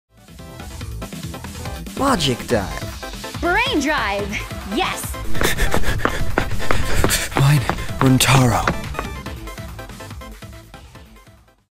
MIND RUNTARO [Danganronpa V3 Comic Dub]
Another something silly!
Voices:
Hajime Hinata - Aidan
Kaede Akamatsu - Atsuyo
Rantaro Amami - Roo

♬ = We post exclusive content on here we don't publish on YouTube!